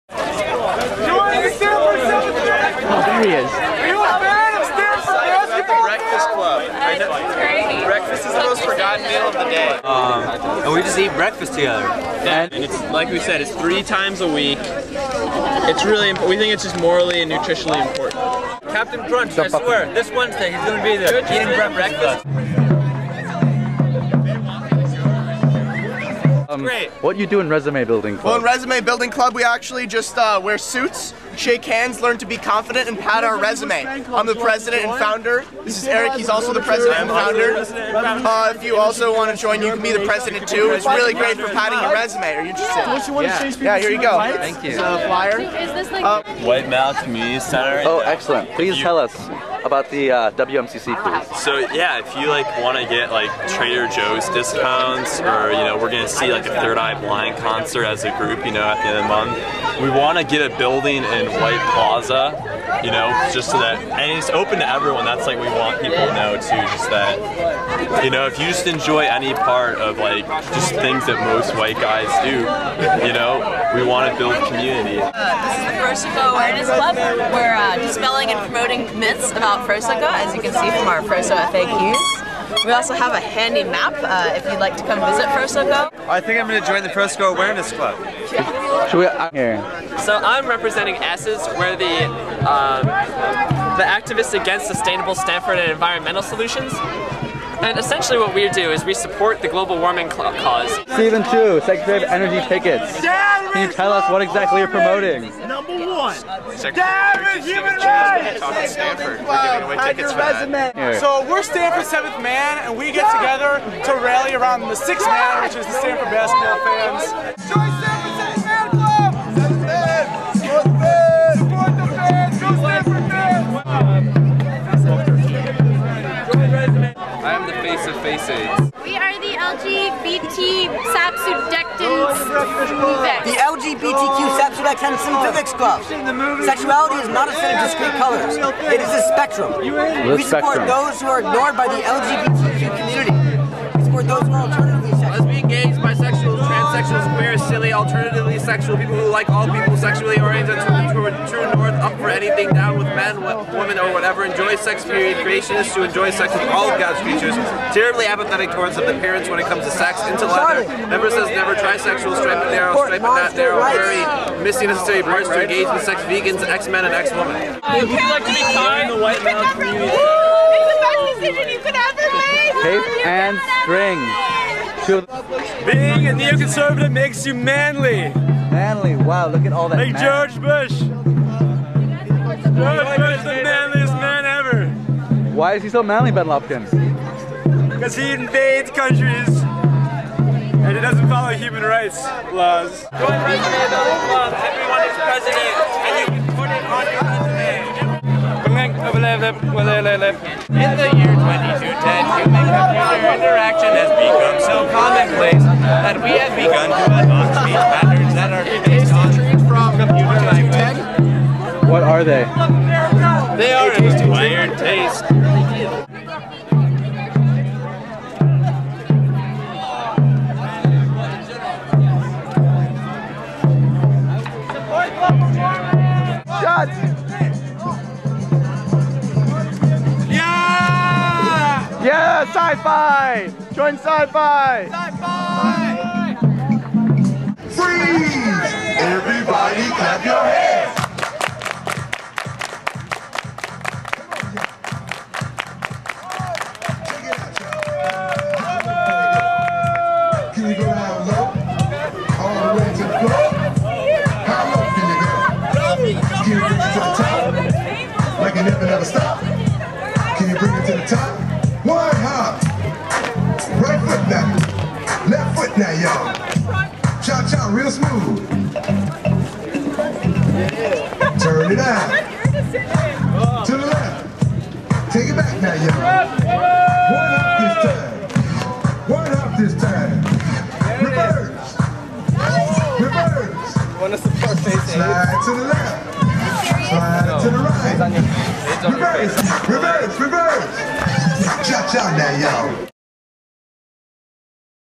Stanford Flipside Activities Fair
Tabling for some of our favorite groups:
Stanford Seventh Man
Resume Building Club
Breakfast Club
AIDS Groups (FACE AIDS, KOOL AIDS, HEARING AIDS)
Psi Phi
ASSES (Pro-Warming)
Class of 2210 - Temporal Study Abroad
Club You Cant Leave
Golf Club
Stanford Women in Engineering
George Bush Fan Club
FroSoCo Awareness
White Men's Community Center
LGDBTDSWEDLKUPQR